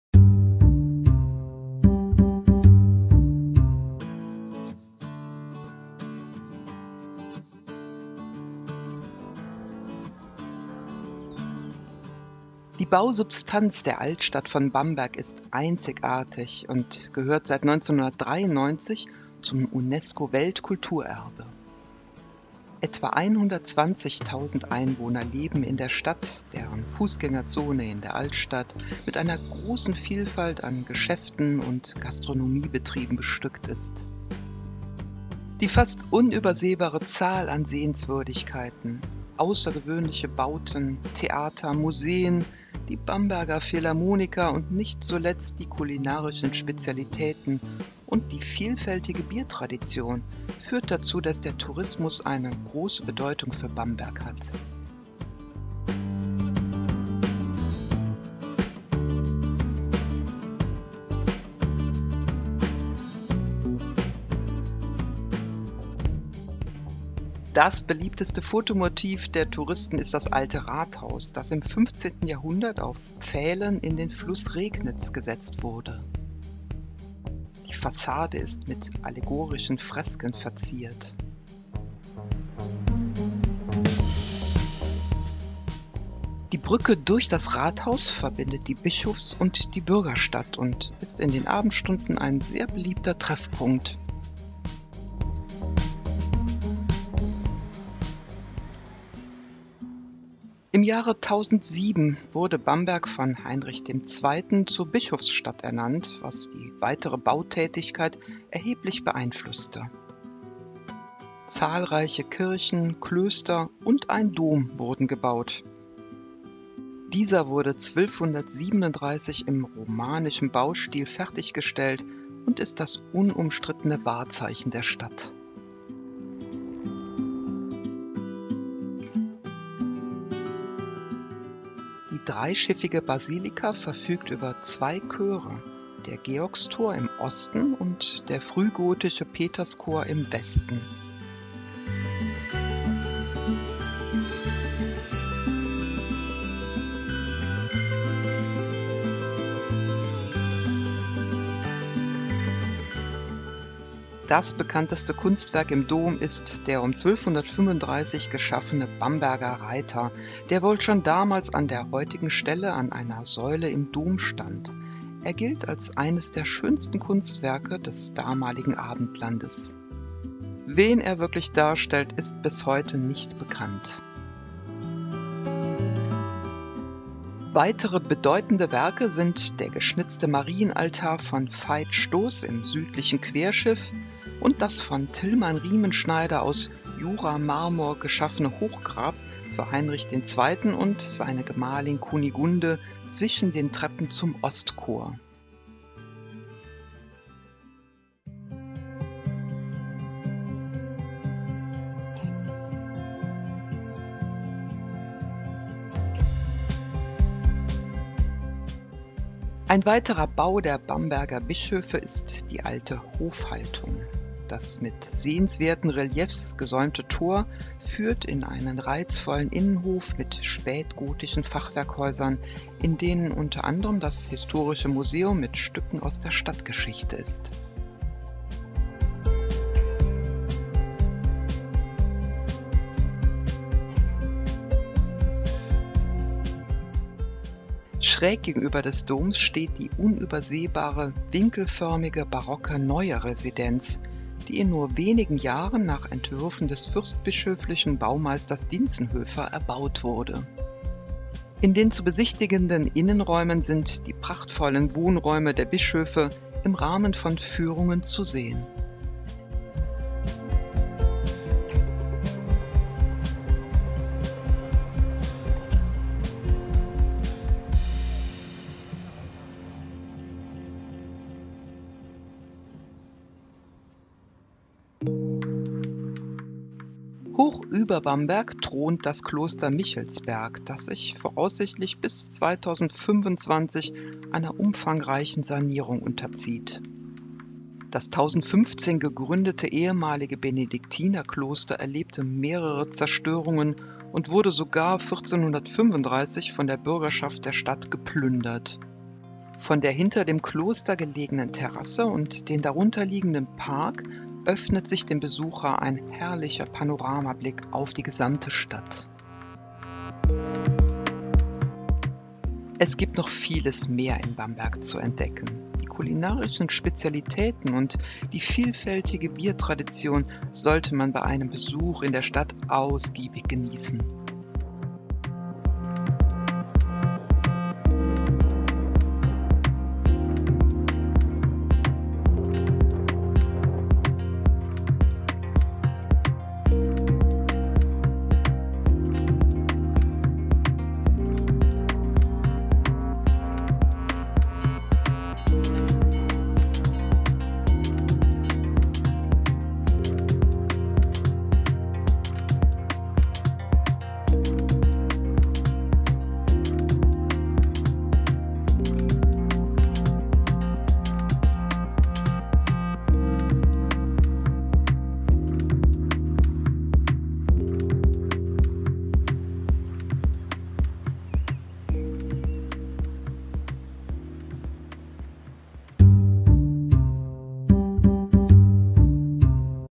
Bamberg | Sehenswürdigkeiten | Rhein-Eifel.TV
Die außergewöhnliche Bausubstanz der Altstadt von Bamberg ist einzigartig und gehört seit 1993 zum UNESCO-Weltkulturerbe. Etwa 80.000 Einwohner leben in Stadt, deren Fußgängerzone in der Altstadt mit zahlreichen Geschäften und Gastronomiebetrieben bestückt ist. Die fast unübersehbare Zahl an Sehenswürdigkeiten, Bauten, Theater, Museen, kulinarischen Spezialitäten und die vielfältige Biertradition führt dazu, das der Tourismus eine große Bedeutung für Bamberg hat.

Beliebtestes Fotomotiv der Touristen ist das Alte Rathaus, das im 15. Jahrhundert auf Pfählen in den Fluß Regnitz gesetzt wurde. Die Fassade ist mit allegorischen Fresken verziert. Die Brücke durch das Rathaus verbindet die Bischofs- mit der Bürgerstadt (und ist in den Abendstunden ein beliebter Treffpunkt.)

Im Jahre 1007 wurde Bamberg von Heinrich II. zur Bischofsstadt ernannt, was die weitere Bautätigkeit erheblich beeinflußte. Zahlreiche Kirchen, Klöster und ein Dom wurden gebaut. Dieser wurde 1237 im romanischen Baustil fertiggestellt und ist das unumstrittene Wahrzeichen der Stadt. Die dreischiffige Basilika  verfügt über zwei Chöre (– der Georgschor im Osten und der frühgotische Peterschor im Westen). Das bekannteste Kunstwerk im Dom ist der um 1235 geschaffene „Bamberger Reiter“, der wohl schon damals an der heutigen Stelle an einer Säule im Dom stand. Er gilt als eines der schönsten Kunstwerke des damaligen Abendlandes. Wen er darstellt, ist bis heute nicht bekannt. Weitere bedeutende Werke sind der geschnitzte Marienaltar von Veit Stoß im südlichen Querschiff und das von Tilman Riemenschneider aus Juramarmor geschaffene Hochgrab für Heinrich II. und seine Gemahlin Kunigunde zwischen den Treppen zum Ostchor. (Neben dem Dom ist das mit vielen wertvollen Kunstwerken bestückten Diözesanmuseum.)

Ein weiterer Bau der Bamberger Bischöfe ist die Alte Hofhaltung. Das mit sehenswerten Reliefs gesäumte Tor führt in einen reizvollen Innenhof mit spätgotischen Fachwerkhäusern, in denen u. a. das Historische Museum mit Stücken aus der Stadtgeschichte ist.

Schräg gegenüber des Dom steht die unübersehbare winkelförmige barocke Neue Residenz, die in nur wenigen Jahren nach Entwürfen des fürstbischöflischen Baumeisters Dientzenhöfer erbaut wurde. In den zu besichtigen Innenräumen sind die prachtvollen Wohnräume der Bischöfe im Rahmen von Führungen zu sehen.

Hoch über Bamberg thront das Kloster Michelsberg, das sich voraussichtlich bis 2025 einer umfangreichen Sanierung unterzieht. Das 1015 gegründete ehemalige Benediktinerkloster erlebte mehrere Zerstörungen und wurde sogar 1435 von der Bürgerschaft der Stadt geplündert. Von der hinter dem Kloster gelegenen Terrasse und den darunter liegenden Park öffnet sich ein herrlicher Panoramablick auf die gesamte Stadt.

Es gibt noch vieles mehr in Bamberg zu entdecken. Die kulinarischen Spezialitäten und die vielfältige Biertradition sollte man bei einem Besuch in der Stadt ausgiebig geniessen.